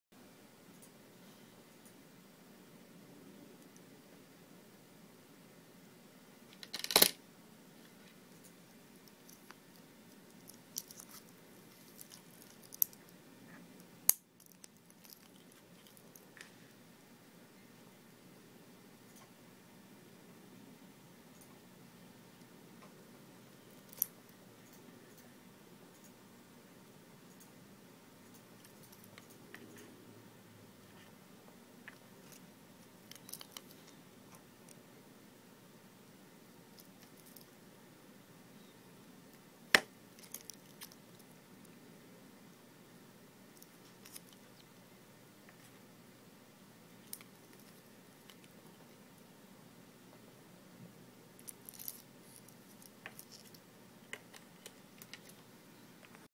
Women's DKNY Crystallized Mother Of Pearl Watch NY8098
- Women's DKNY Crystallized Mother Of Pearl Watch NY8098. This model comes with features such as
Donna Karan New York watch collection.
Stainless steel case.
White plastic bracelet.
Mother of pearl dial.
Crystallized bezel.
Chronograph function.
Date display.
Scratch resistant mineral crystal.
Water resistant up to 50 meters/ 165 Feet.
Case size: 38 mm.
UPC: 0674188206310
Enjoy this video review.